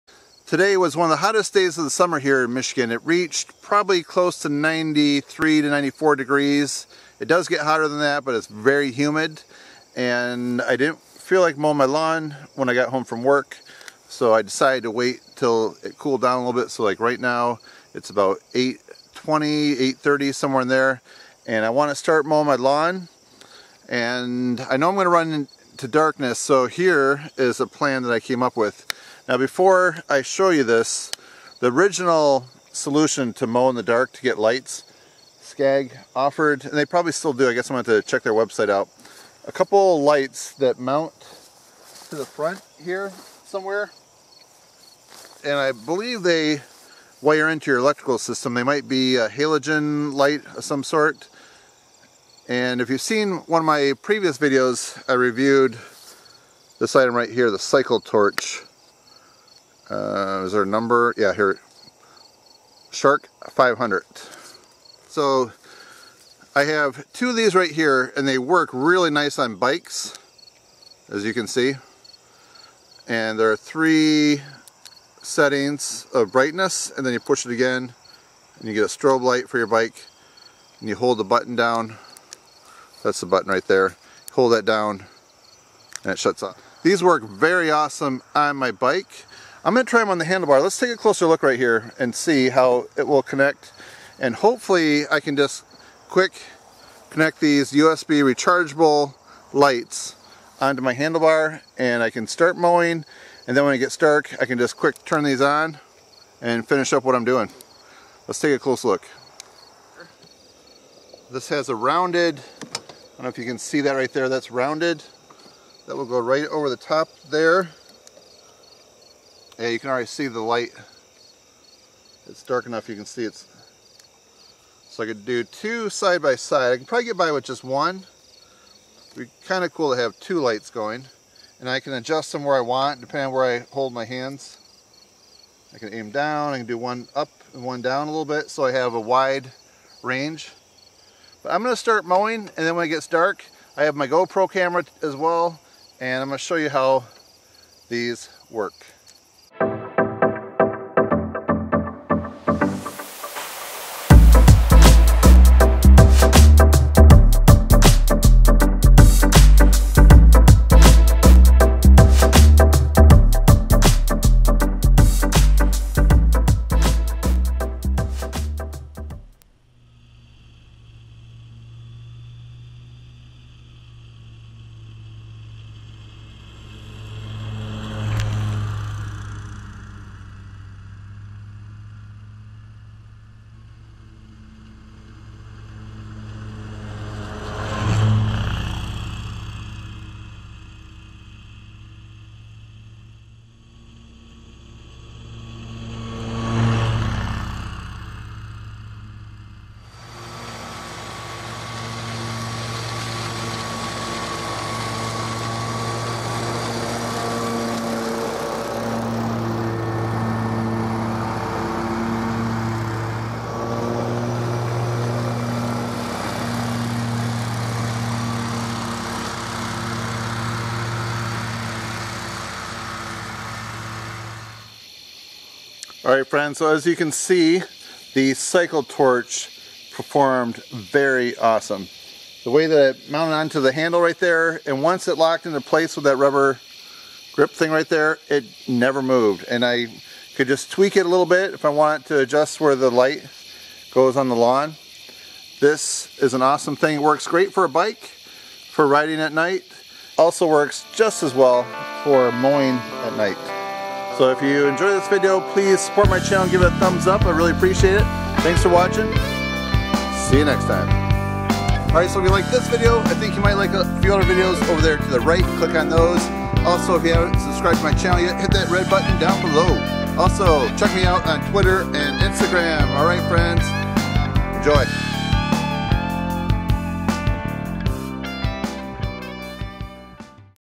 NIGHTTIME LAWN MOWING WITH LED BIKE LIGHTS ON A ZTR
I need to mow at night once in awhile, so I decided to try out the rechargable LED bike lights by the Cycle Torch Company.  See the Shark500 lights in action as I mow in the dark night sky.  I compared the cost of the Scag Halogen Light Kit and it costs $220, compared to these portable lights costing $80.  I only mow at night on a seldom basis and don't have any neighbors close to me, so the noise isn't an issue at night.